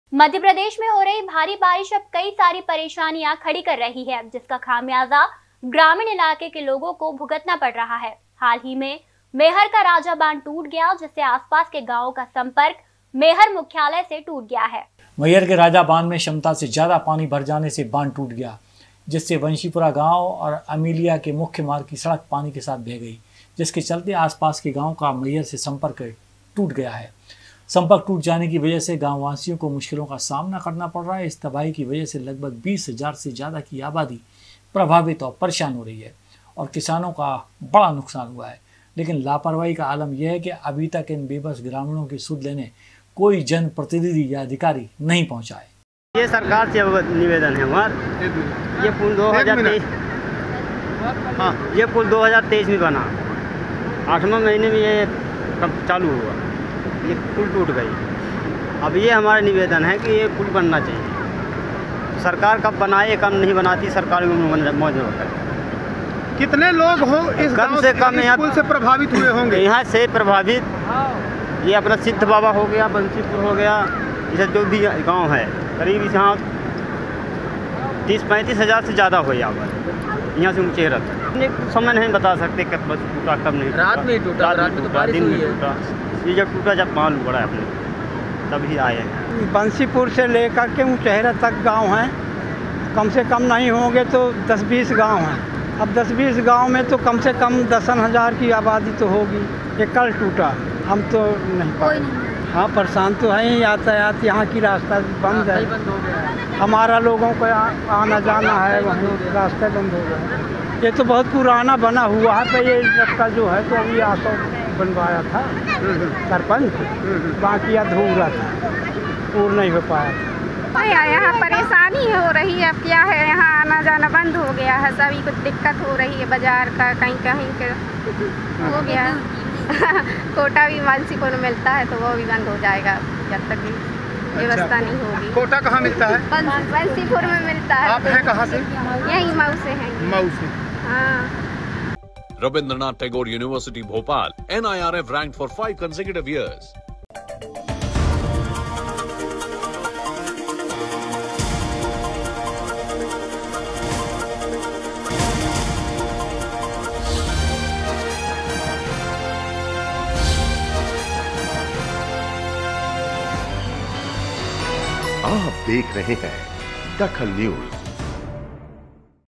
DAKHAL NEWS जलभराव से टूटा मैहर का राजा बांध
जलभराव से टूटा मैहर का राजा बांध
कई गाँवो का मैहर से टूटा संपर्क